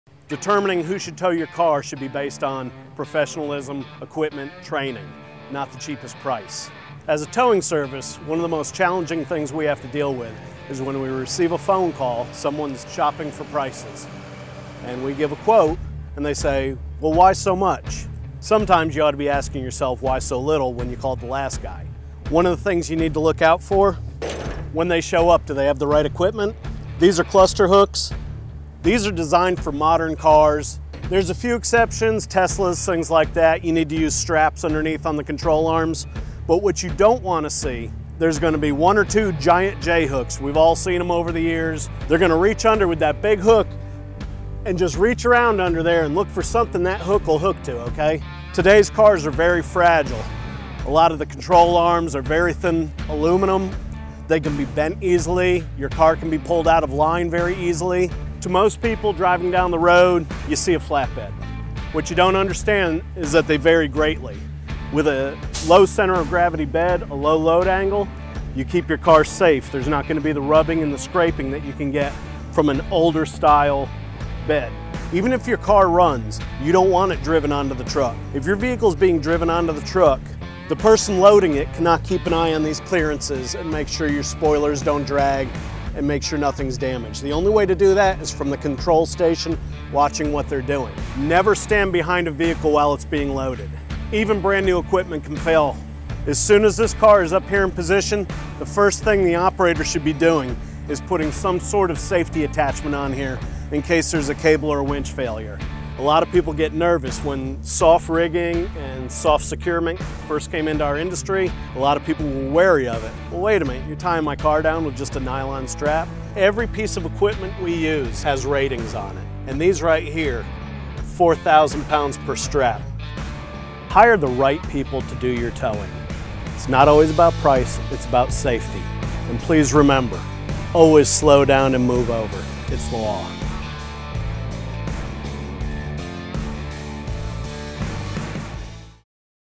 The Right Way To Tow
When motorists are on the side of the road, it is not uncommon for them to be shopping around for tow services. Typically, price is the highest priority. Price is not everything when it comes to a towing service. Getting the cheapest price will likely mean that you are getting what you pay for: cheap service. At Southside Wrecker, we have over 30 years of experience towing cars properly and promptly. You can feel confident that your car will not be damaged while it is in our care. There are a few warning signs to look for when determining if you have found a professional tow service.

Their Equipment—Modern cars are more fragile than you may realize. If the tow service does not use the right equipment, your car could be damaged in the hauling process. Cluster hooks are designed to safely attach to your car without causing damage below the surface or knocking your car out of alignment. If the tow service shows up with J hooks to tow your car, dismissing their service would be wise. You will likely be paying more later to fix your car from the use of incorrect securing methods.

Their Truck—Older tow trucks tend to have steeper load angles which can cause scraping and damage to your vehicle while loading and unloading. Your vehicle should be winched on and off the bed even if it runs. The trucks we use at Southside Wrecker utilize the latest advancements to load your car without putting a strain on your vehicle.

Their Safety Attachments—When a car is loaded on the tow truck, your tow service should be securing the vehicle with at least four points between the bed and the vehicle. Some motorists are wary of securing a vehicle with straps. Straps can be perfectly acceptable for securing your vehicle if they are rated for the appropriate weight. In many cases nylon straps are rated stronger than chain. Most manufacturers now prefer wheel straps to be used. Never stand behind a tow truck while your vehicle is being loaded or unloaded.

When you need a tow service, call Southside Wrecker. Our tow services are high quality, speedy, and fairly priced.